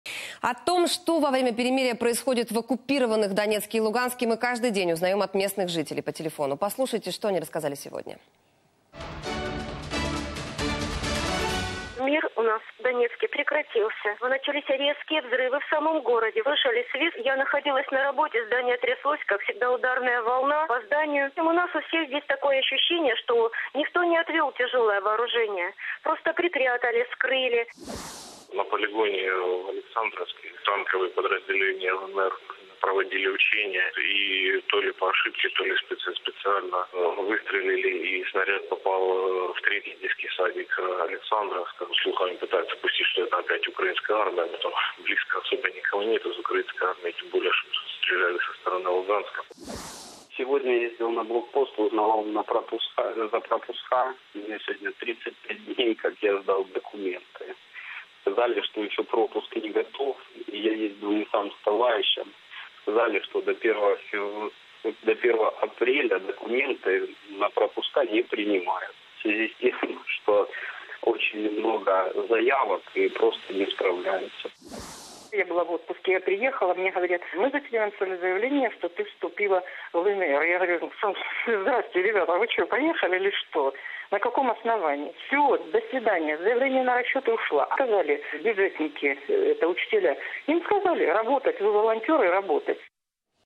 Танк выстрелил в детский сад на учениях в ЛНР
"На полигоне Александровский  танковые подразделения ЛНР проводили учения, и то ли по ошибке, то ли специально выстрелили - и снаряд попал в 3тий детский садик Александровска. Слухами пытаются пустить, что это опять украинская армия, но там и близко и никого нет из украинской армии тем более, что стреляли со стороны Луганска", - говорит житель Луганска, программист Сергей.